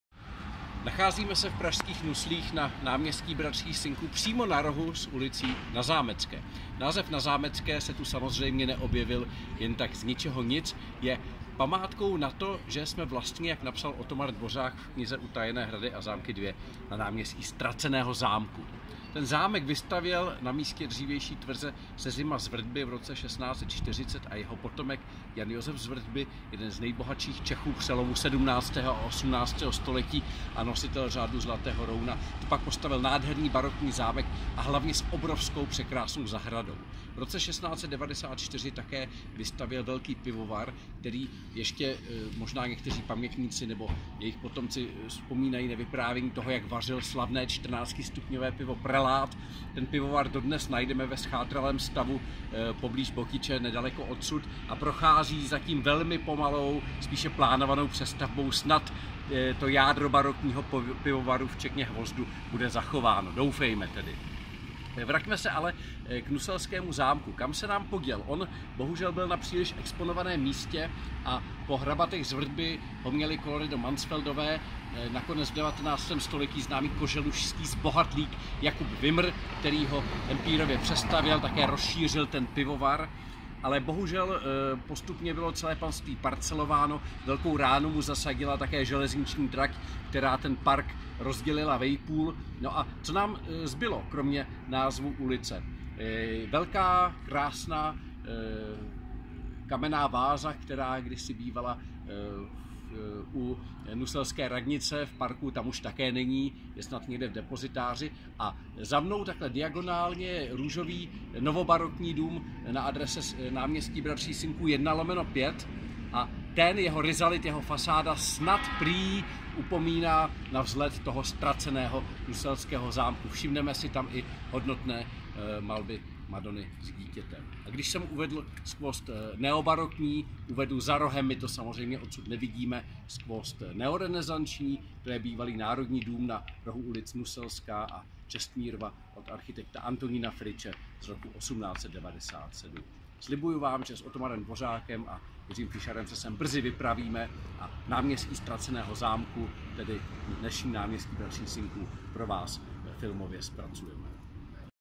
Náměstí ztraceného zámku aneb pátrání po nuselském zámku na náměstí Bratří Synků
Náměstí Bratří Synků, Praha 4 - Nusle, a zaniklé i dochované pozoruhodnosti v jeho okolí. Natočil Josef Snětivý ml. dne 10. května 2018.